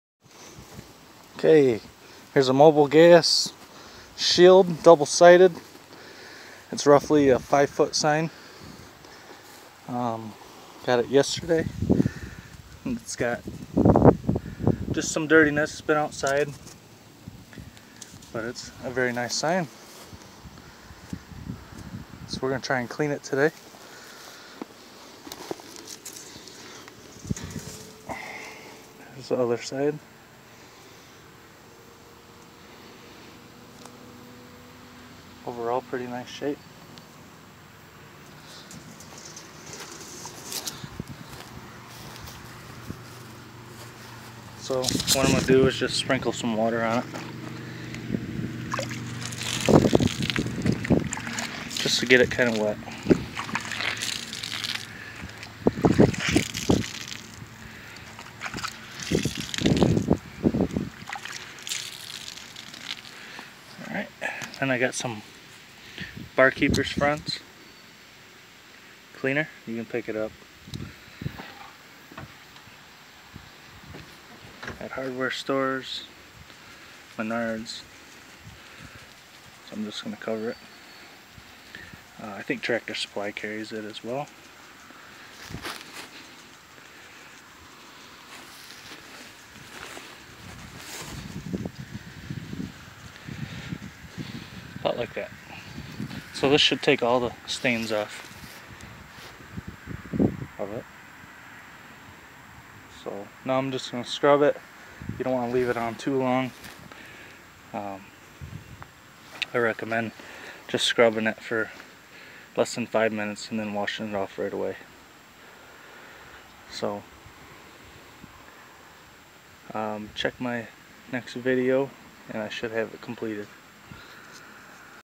How to clean a mobilgas sheild porcelain sign bar keepers friends
Cleaning a mobilgas porcelain sign using bar keepers friends. See my next video for results